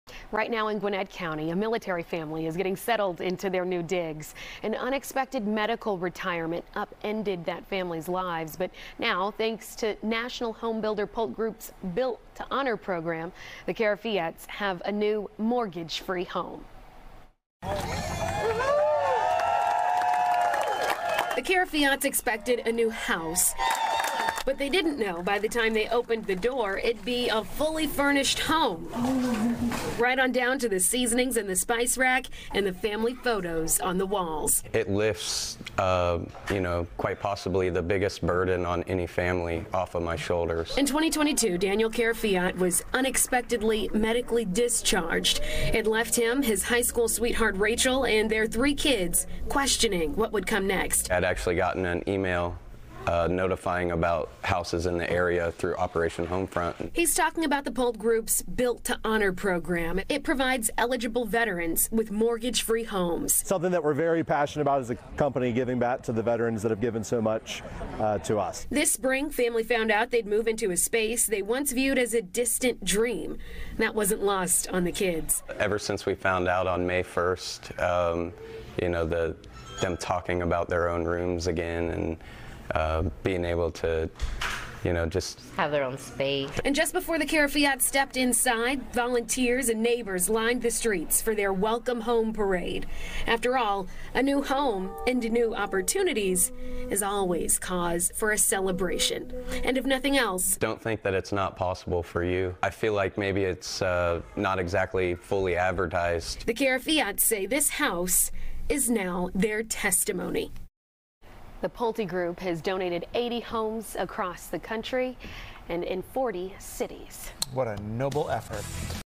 Military family gifted mortgage-free home | FOX 5 News
A military family in Gwinnett County is settling into their brand new home. After an unexpected medical retirement upended the family's lives, the National Homebuilder Pultegroup's Built to Honor Program jumped right in to help the Karafiat family out. Their new home is mortgage-free.